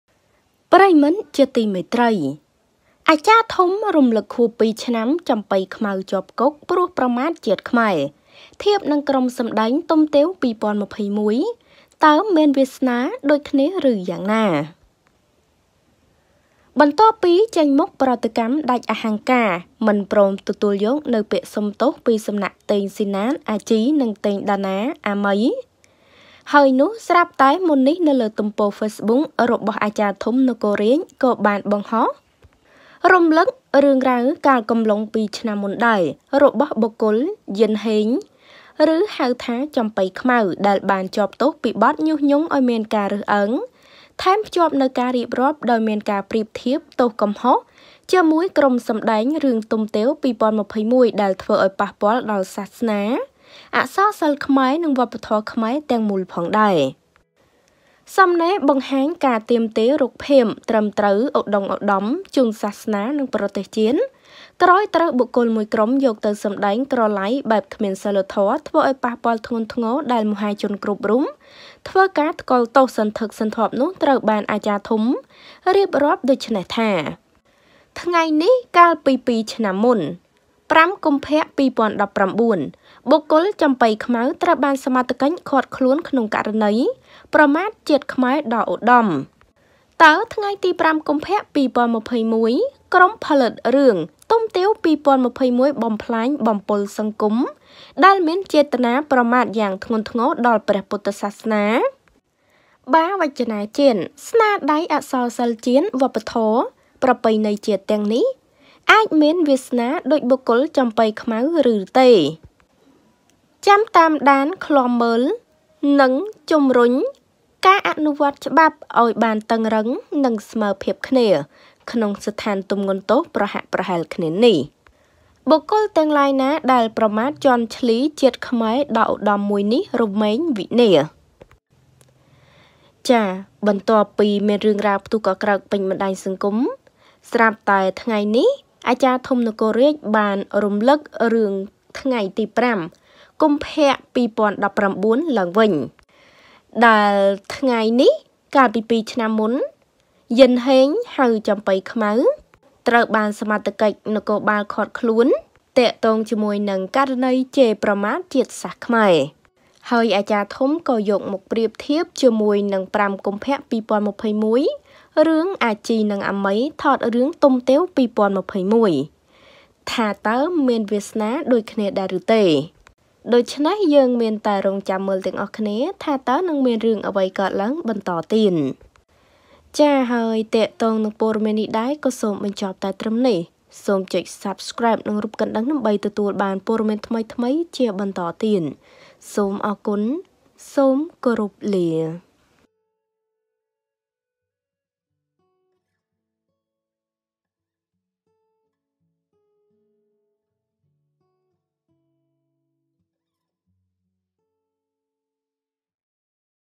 អាចារ្យធំ រឮកខួប២ឆ្នាំ ចំប៉ីខ្មៅ ជាប់គុក ធៀបនឹងក្រុមសម្ដែង «ទុំទាវ ២០២១» តើមានវាសនាដូចគ្នាឬយ៉ាងណា?
សូមអធ្យាស្រ័យ

ចំពោះការមិនបានតប ខំមិនបានគ្រប់គ្នា

Ms Socheata